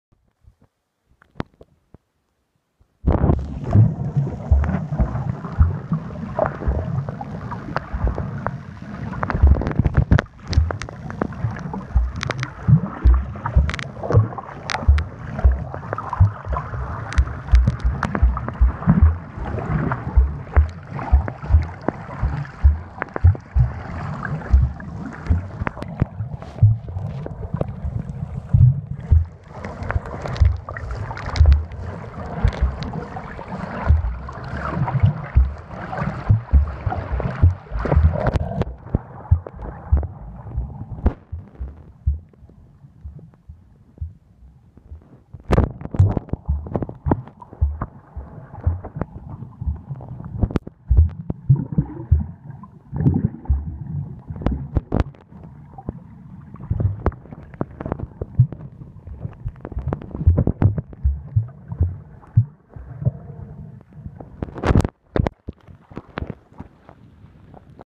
Snorkeling with a turtle at Tamarindo Beach, Culebra PR. with iphone5
Vacations !! July 2015